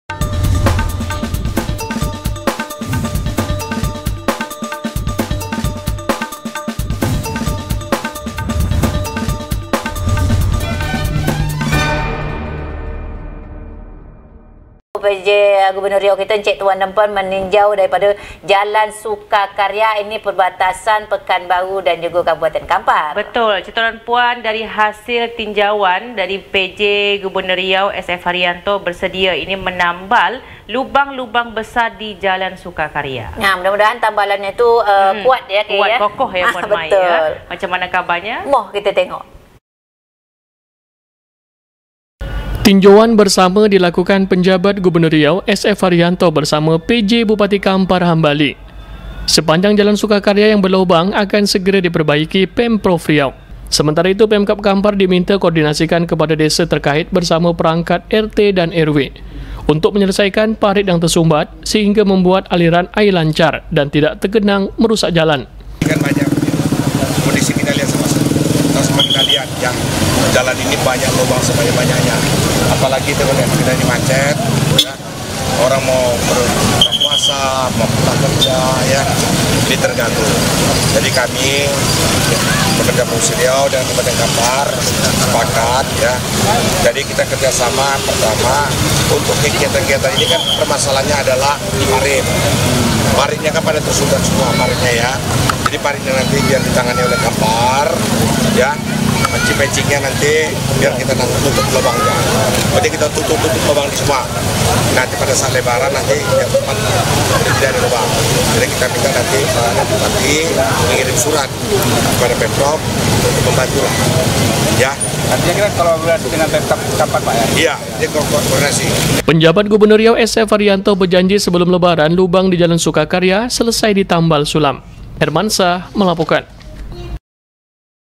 PJ GUBERNUR : JALAN SUKA KARYA RUSAK BERLOBANG MENJELANG LEBARAN AKAN MULUS
Pekanbaru, riautelevisi.com- Jalan Suka Karya perbatasan Pekanbaru-Kampar, di tinjau Penjabat Gubernur Riau, bersama PJ Bupati Kampar. Dari hasil tinjauan, PJ Gubernur Riau, SF Hariyanto bersedia menambal lubang-lubang besar di Jalan Suka Karya.

Tinjauan bersama dilakukan Penjabat Gubernur Riau, SF Hariyanto bersama PJ Bupati Kampar, Hambali. Sepanjang Jalan Suka Karya, yang berlobang akan segera diperbaiki Pemprov Riau. Sementara itu, Pemkab Kampar diminta koordinasikan kepada desa terkait bersama Perangkat RT dan RW, untuk menyelesaikan parit yang tersumbat, sehingga membuat aliran air lancar dan tidak tergenang merusak jalan.

Penjabat Gubernur Riau, SF Hariyanto berjanji sebelum lebaran lubang di Jalan Suka Karya, selesai di tambal sulam.